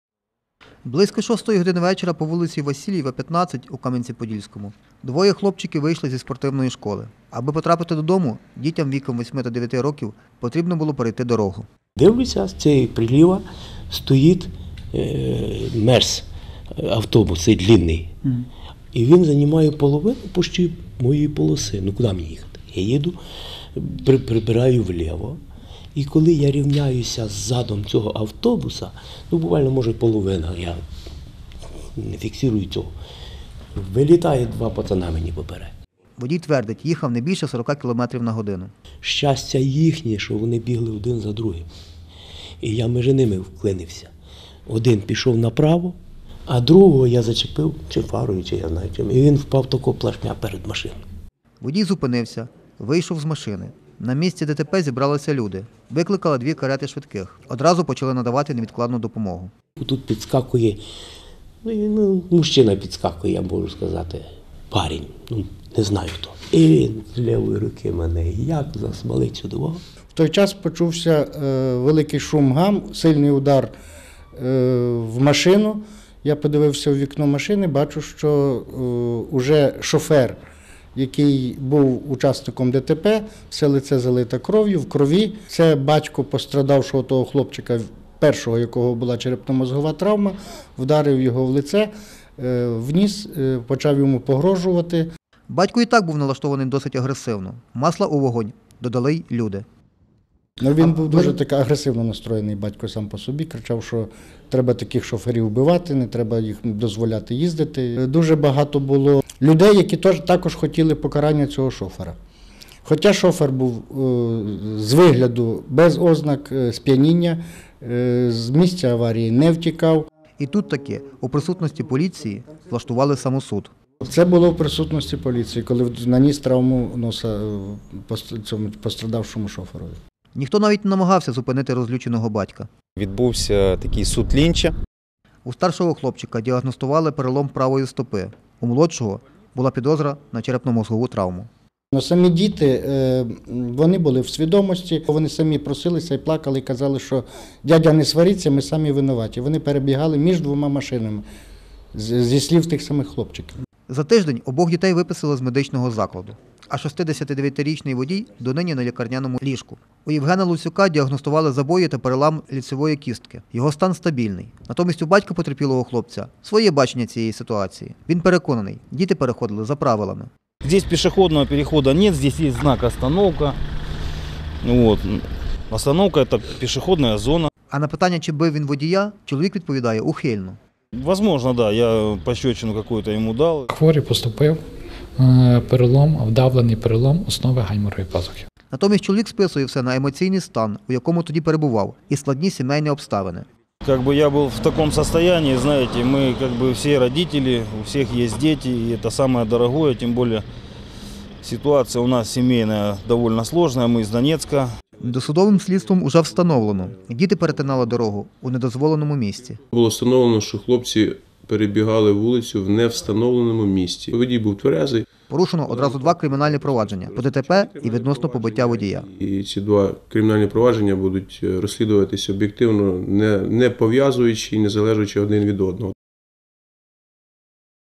У Кам'янці батько побив водія, який збив його дитину
У Кам’янці-Подільському двоє дітей потрапили під колеса автівки. Батько одного з потерпілих, що приїхав на місце ДТП з кулаками накинувся на водія. У результаті і діти і чоловік похилого віку опинилися в лікарні.